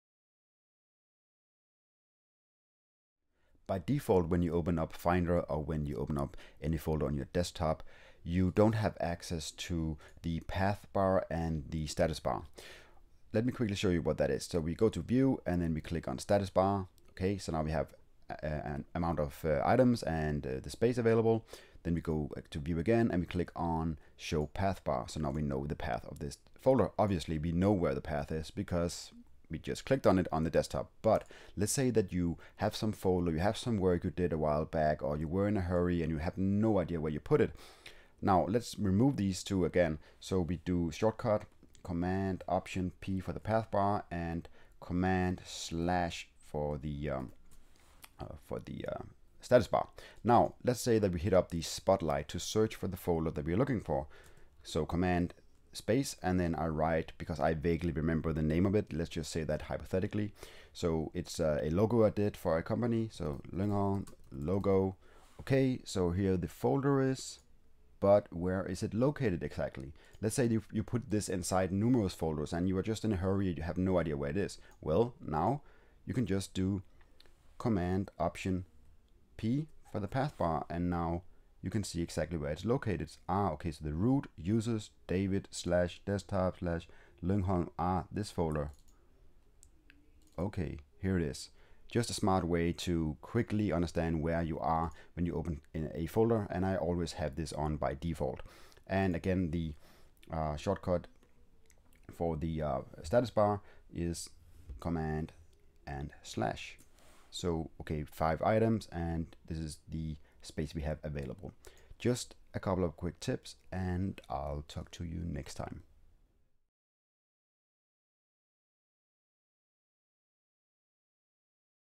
OSX Show Status and Path Bar Inside Finder and Folders (2016)
In this video I show you two practical tips for Mac users:

How to show the status bar, and how to show the path bar inside finder (folders).

Especially showing the path bar is useful if you lose track of where you put some work. If you locate it with the search spot light, you don't get to see the actual path even if you find the folder or file you were searching for, but by enabling the path bar in finder you can always locate the exact directory.

The status bar in finder is useful to see how much space you have available as well as how many files are inside your documents.

The three shortcuts shown in the video:

Show Path Bar: CMD + OPTION + P
Show Status Bar: CMD + Forward Slash (Tab + 7)
Spotlight Search: CMD + Spacebar

Was this helpful to you?
You can say thanks by buying me a coffee — or by liking and commenting on the video. Thank you!